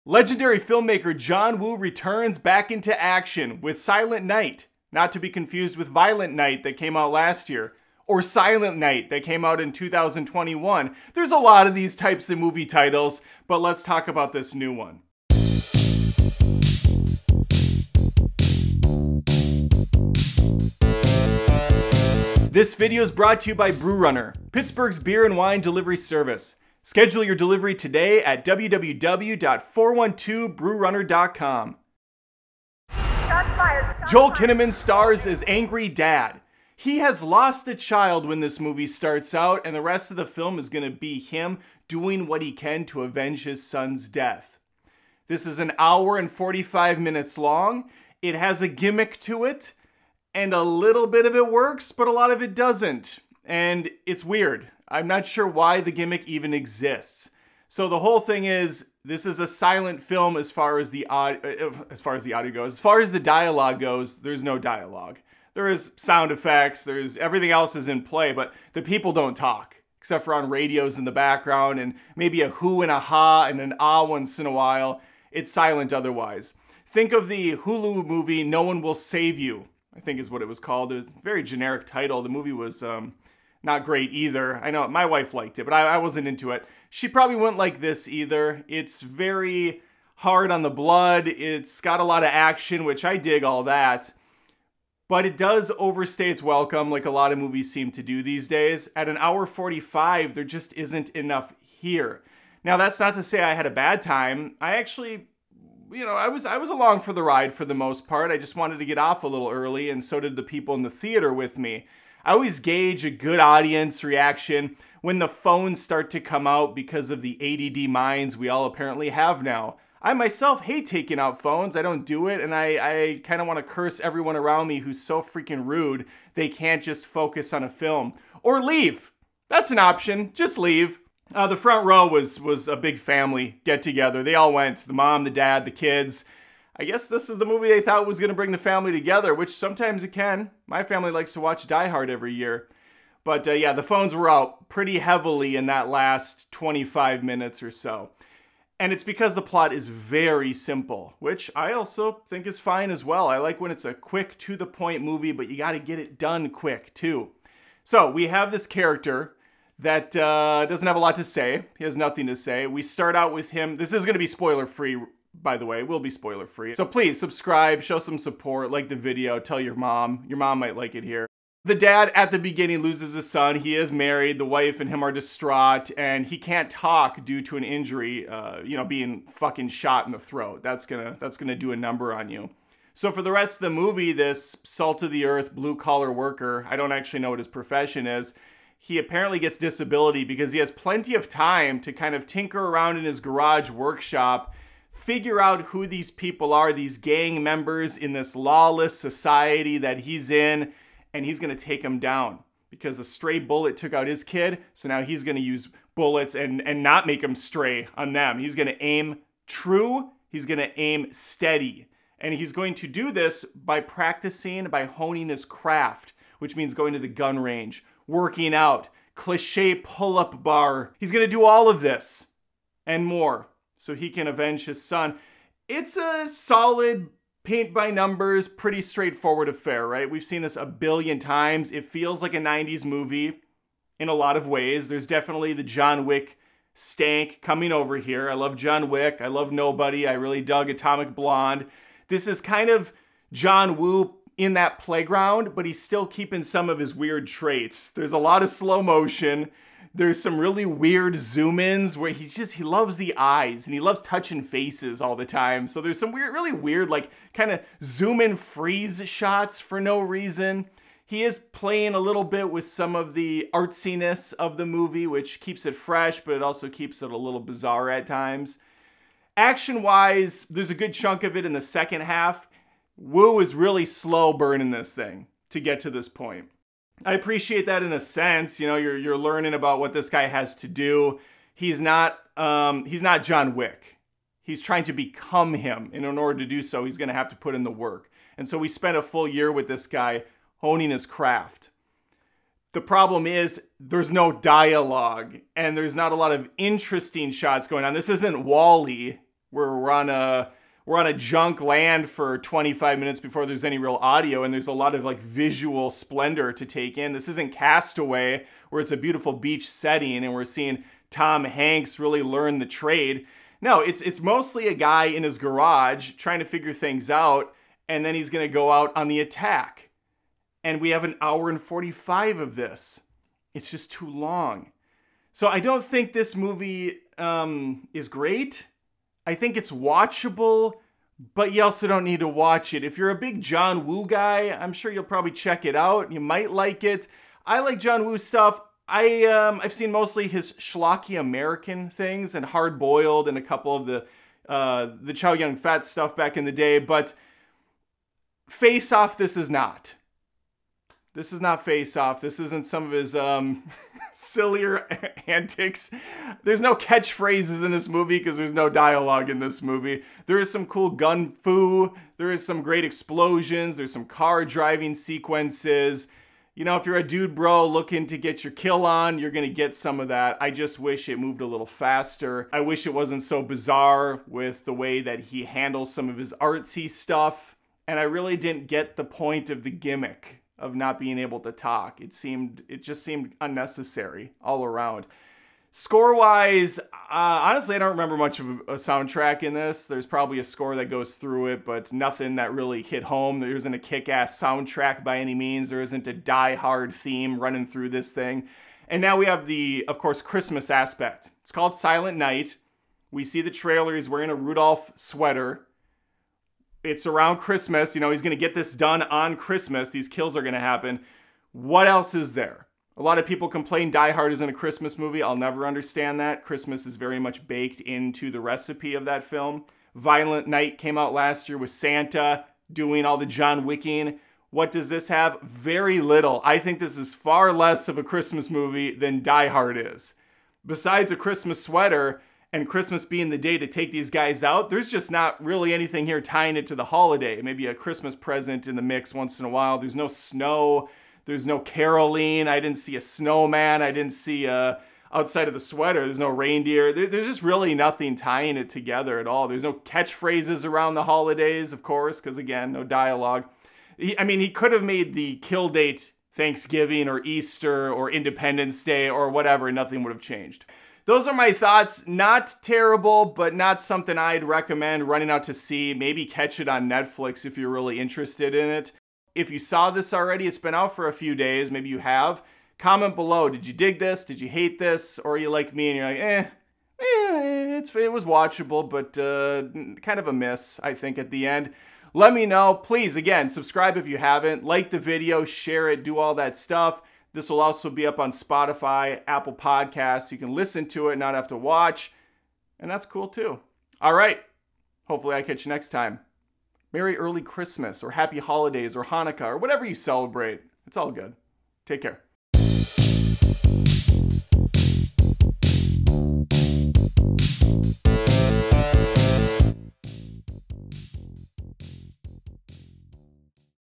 Silent Night Movie Review - More Like Silent Alright
Silent Night movie review. Silent Night is another John Woo action movie that appears inspired by more modern flicks like John Wick. It's also acting like a Christmas movie but Die Hard is far closer to a film for the holidays. While I did enjoy Silent Night, for the most part, I won't deny that the gimmick is pretty lame and brings nothing to the table. In fact, it does the opposite. The thin plot also means the movie is just kinda going through the motions without providing anything fresh. Silent Night ends up just being alright.